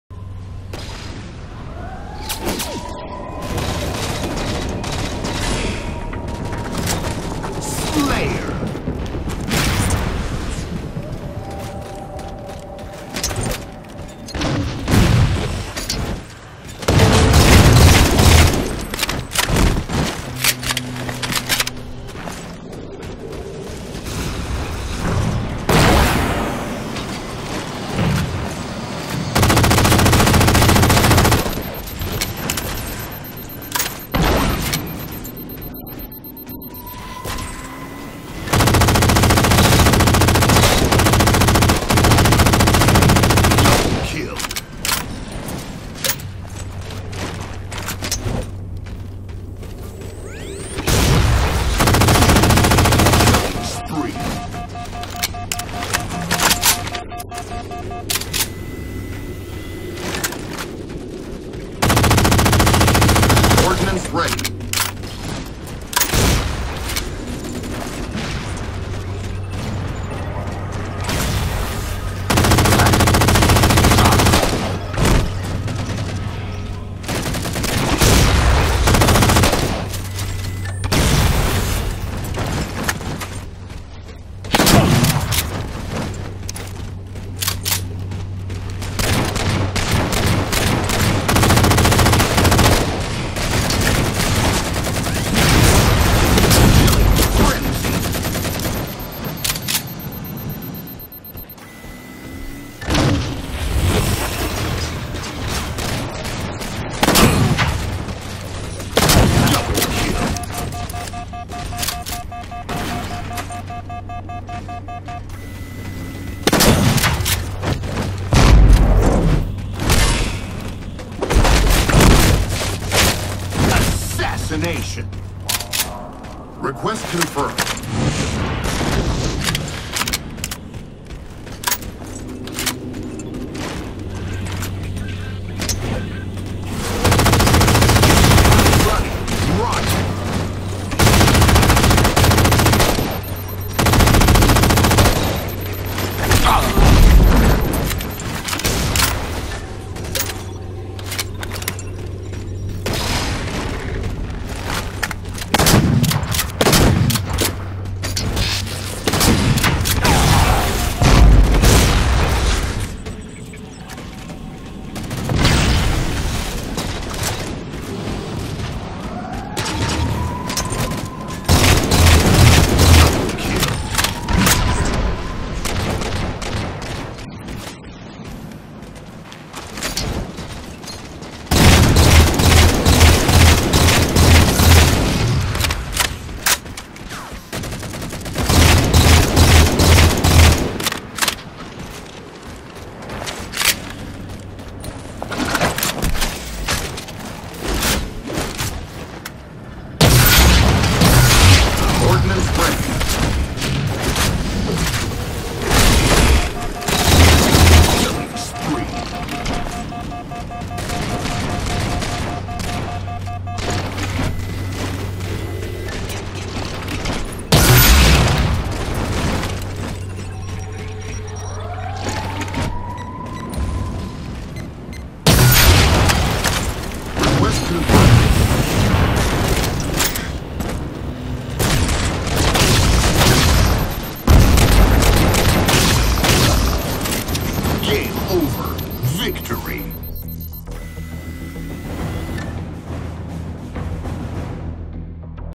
Opening a can of Whoop-ass
Wrapped up a game of FFA in 4 minutes with 25 kills and 1 death.